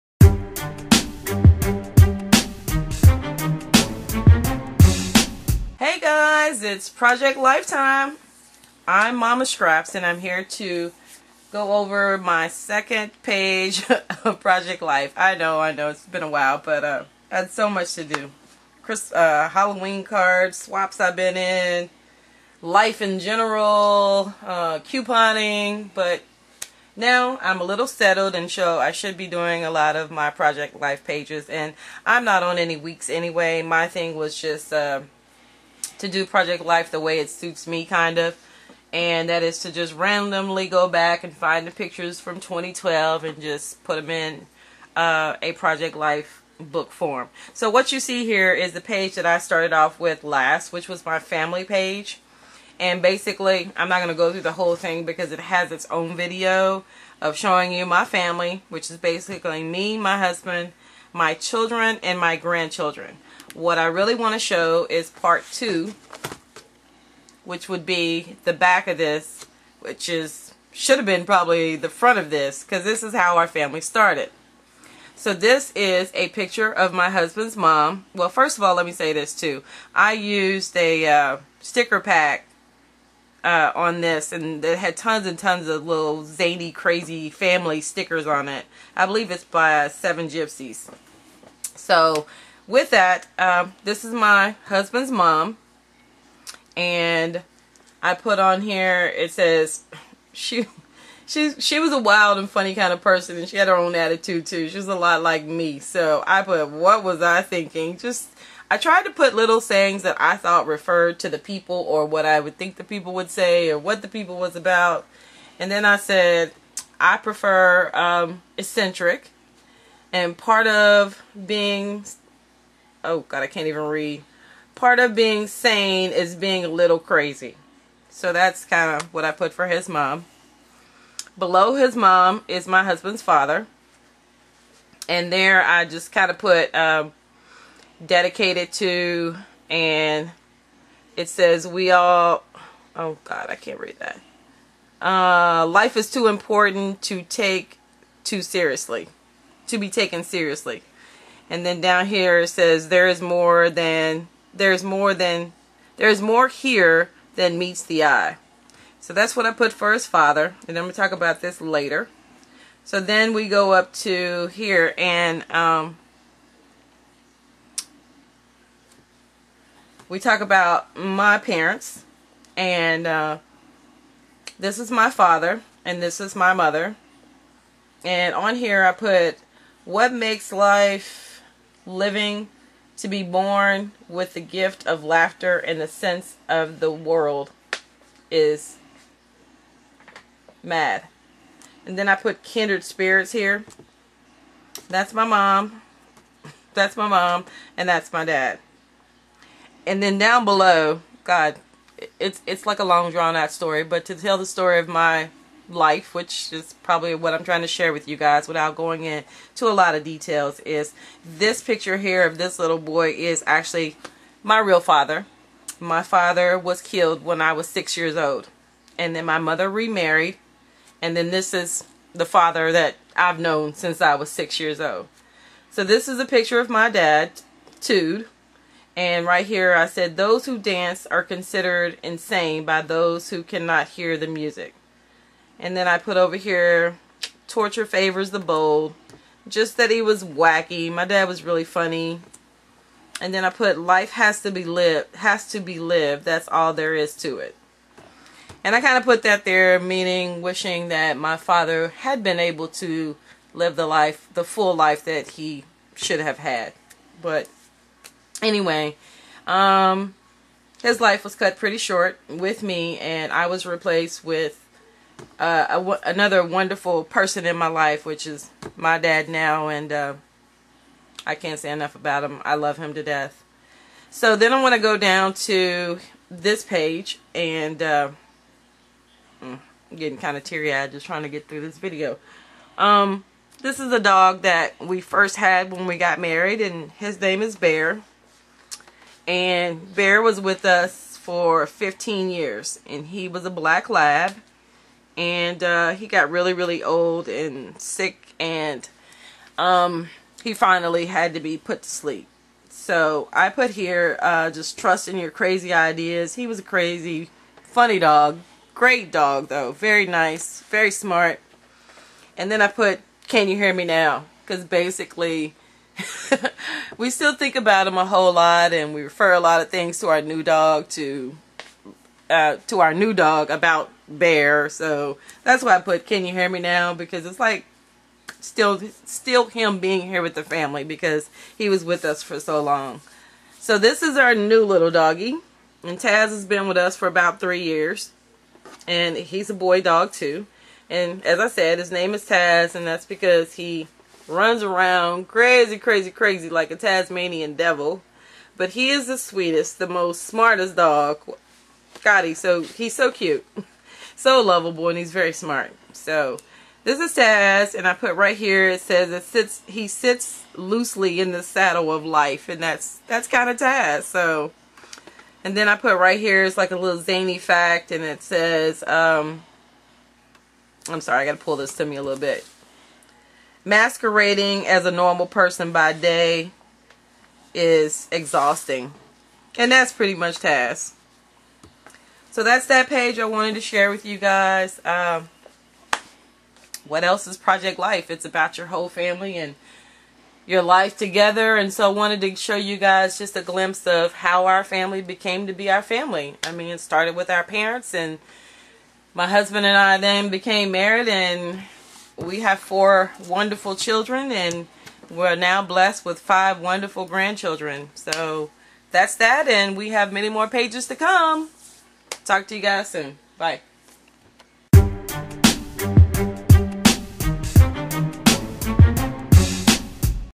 Project Life #2 Family
Here is my back page to my project life introduction page. Introducing you to  the rest of my family.I hope that you all enjoy. I an new at Project life but I am off to a very good start.